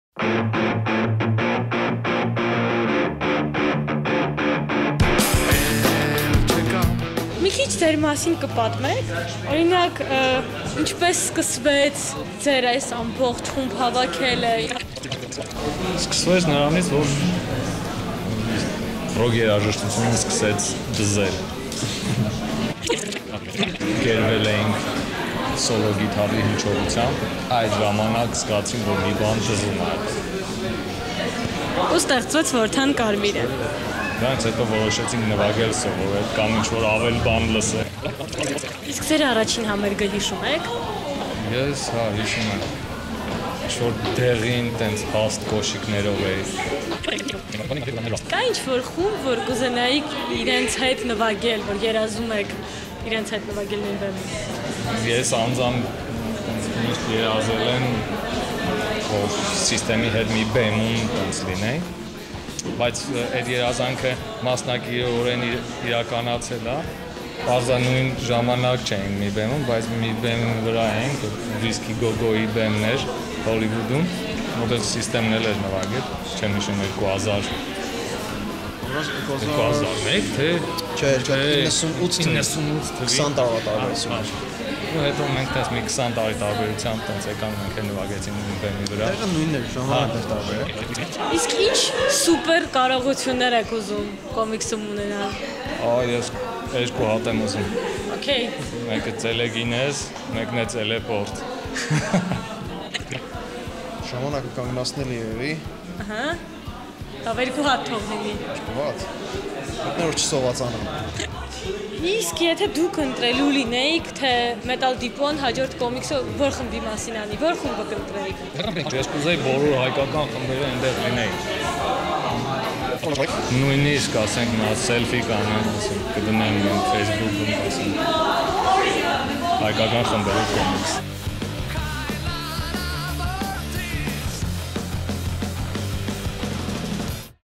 Our good friends from VORDAN KARMIR band at Metal Meets Comics party
At METAL DEPOT we love having guests. Especially bands. Especially, those bands that we love! And if in addition those bands also love comic books, then everybody is happy and we end up having tons of fun! \m/
Stay metal & read epic comic books